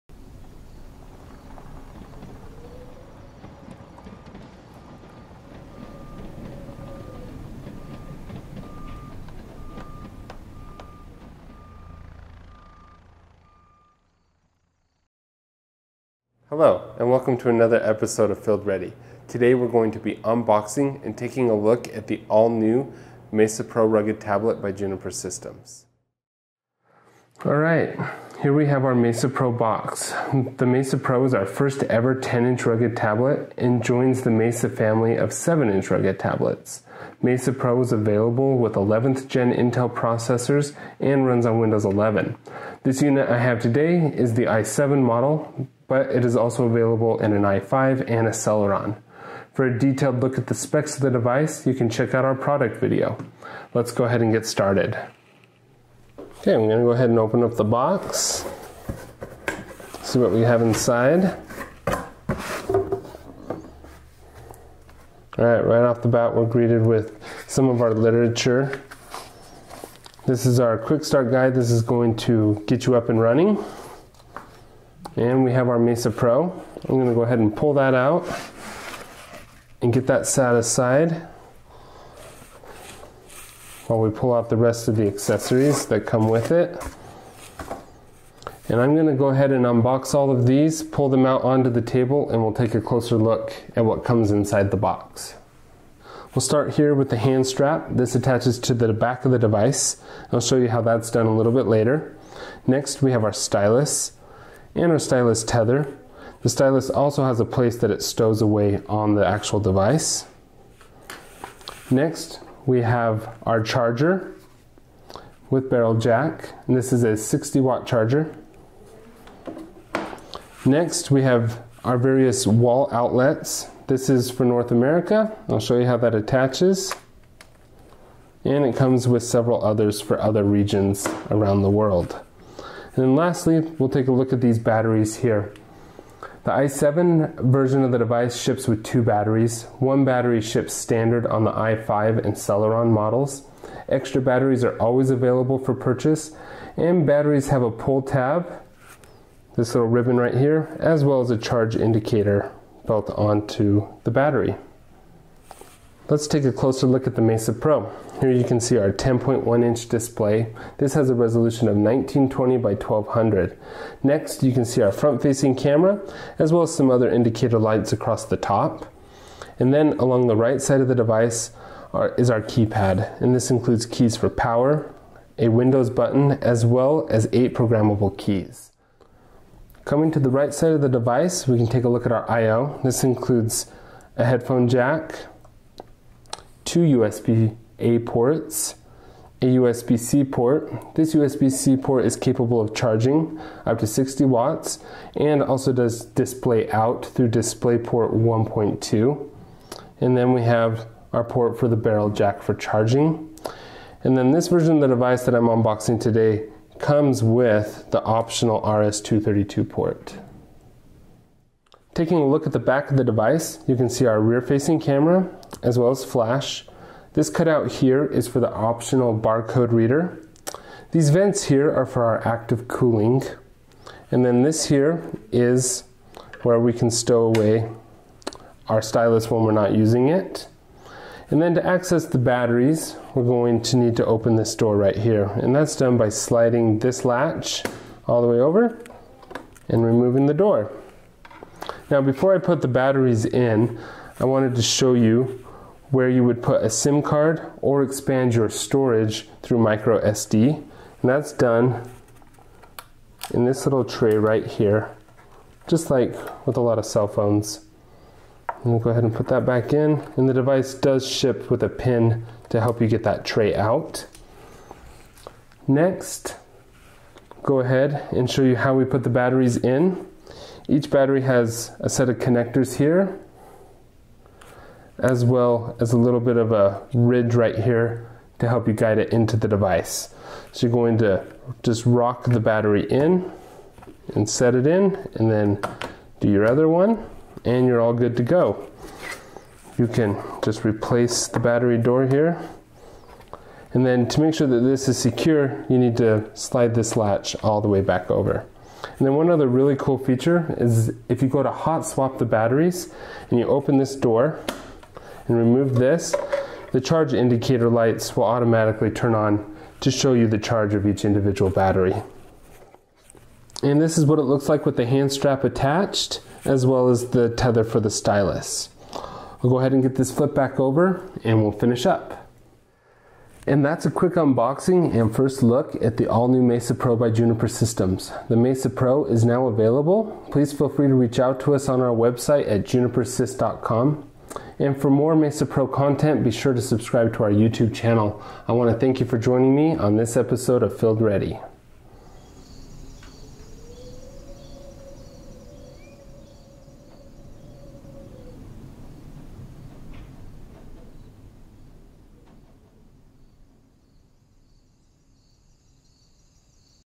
Unboxing the Mesa Pro Rugged Tablet | Field Ready
In this episode of Field Ready we unbox the Mesa Pro Rugged Tablet and take a closer look at what you can expect when you receive your Mesa Pro.
The Mesa Pro is a 10-inch Windows rugged tablet by Juniper Systems. It features 11th Gen Intel processors and an ultra-rugged design.